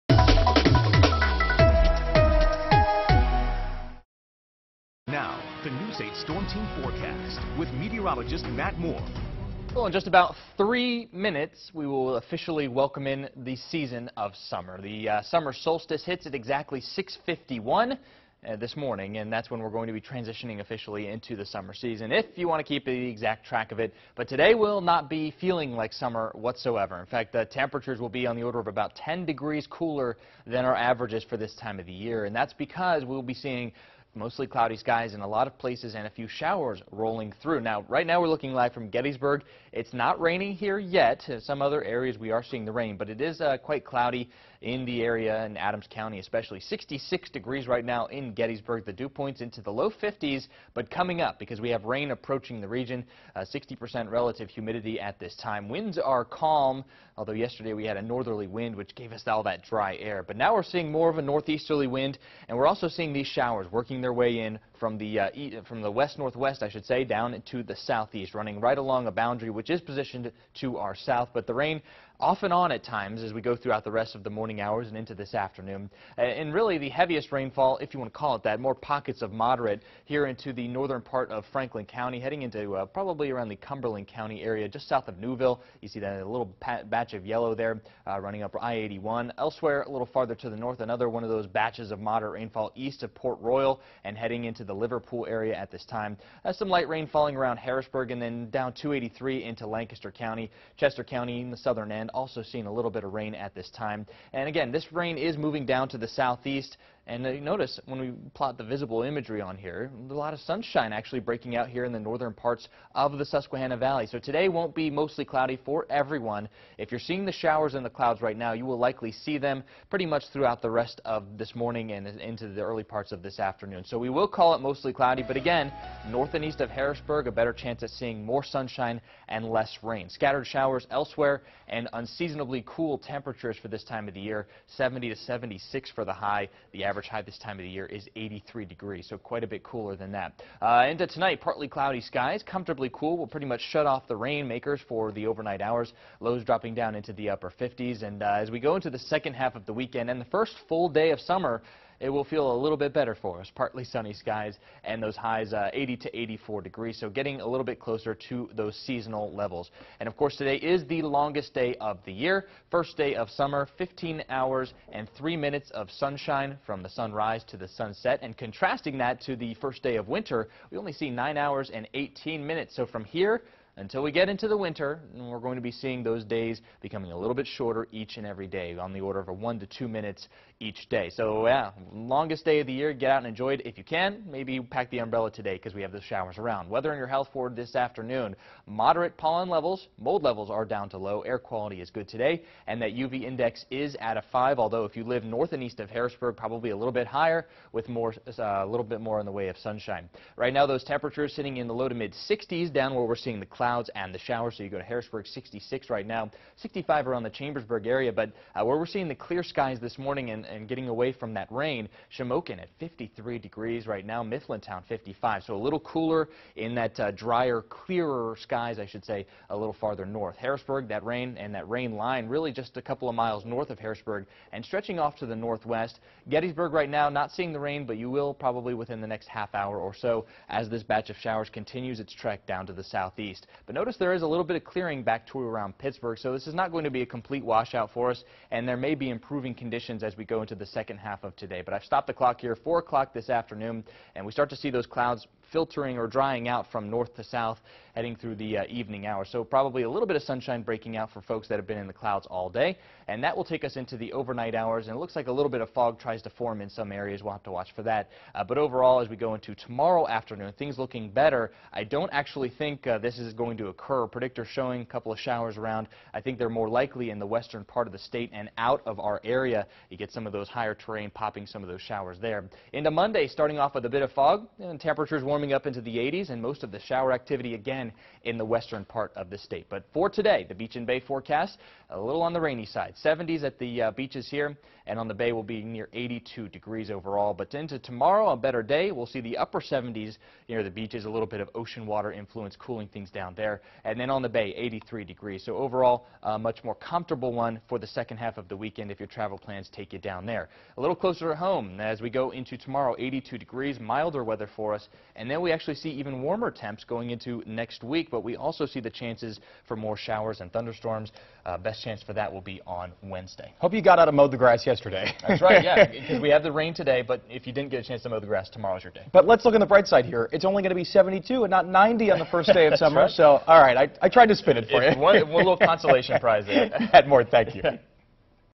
Showers to kick off summer
After a nice end to the work-week, showers have moved back in for the first day of summer!